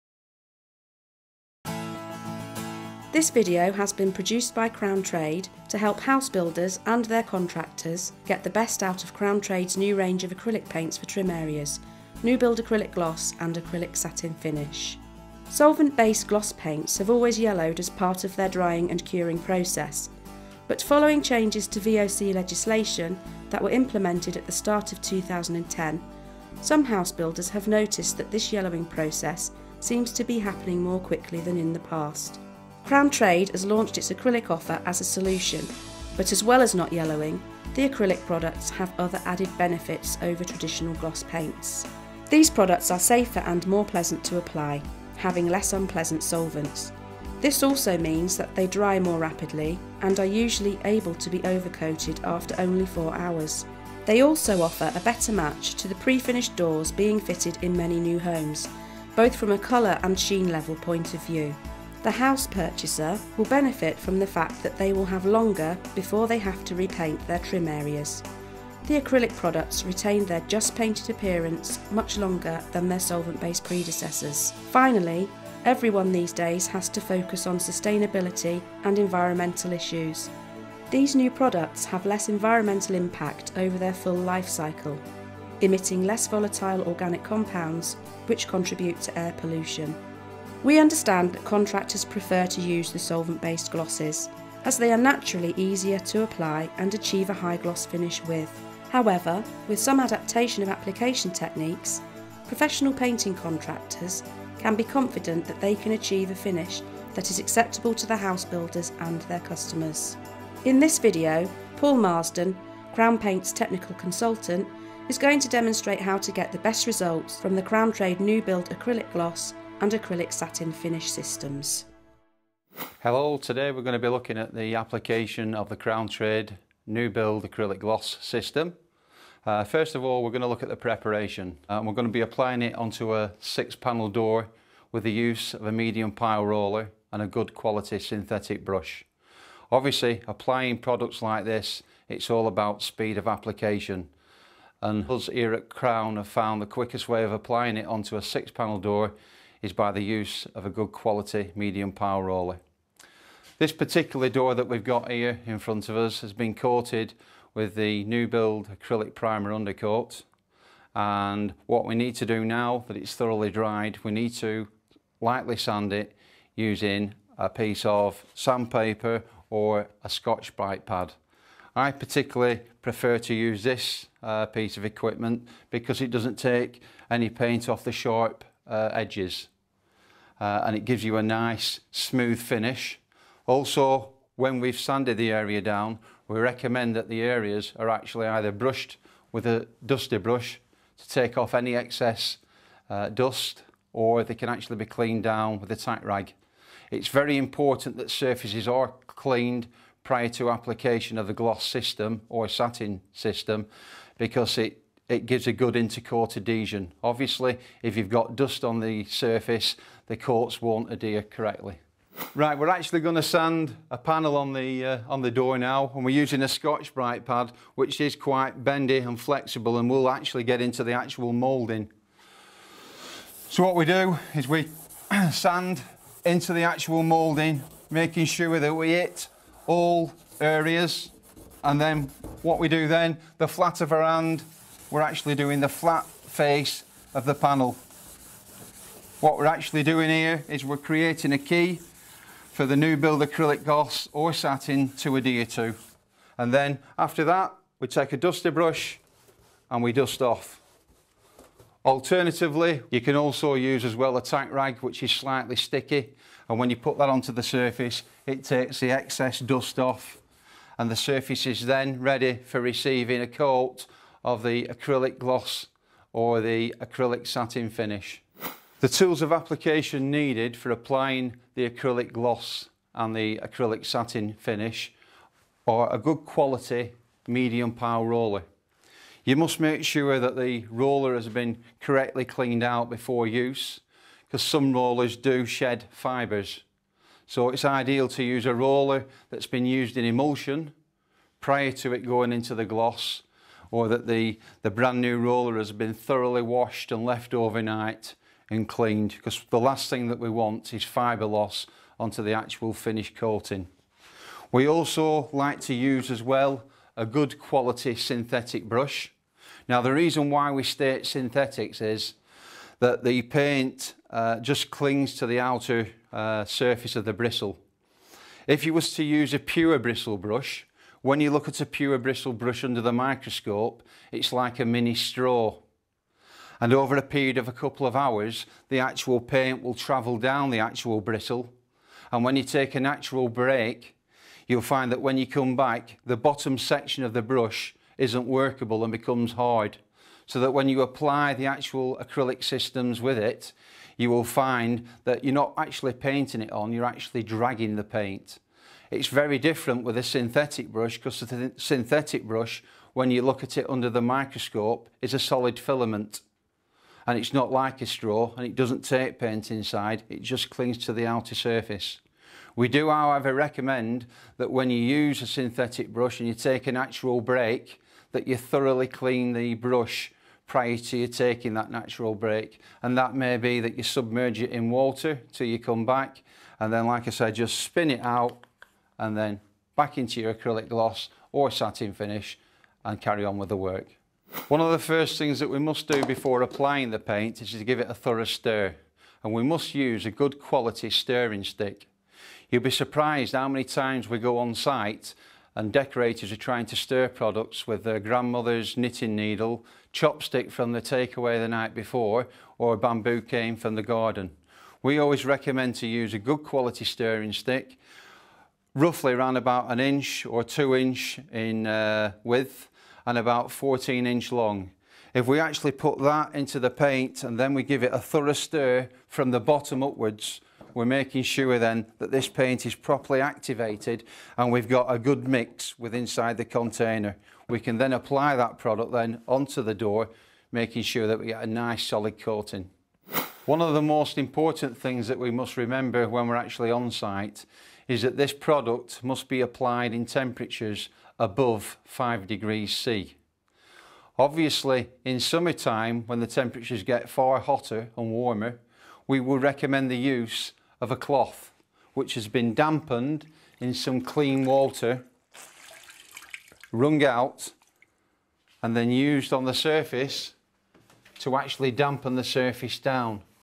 Crown Trade New Build Acrylic Gloss Application 1/2
This video has been produced to help house builders and their contractors get the best out of Crown Trade's new range of Acrylic paints for trim areas, New Build Acrylic Gloss and New Build Satin Finish. Crown Trade has launched its Acrylic offer as a solution to the yellowing process found in solvent based gloss paints.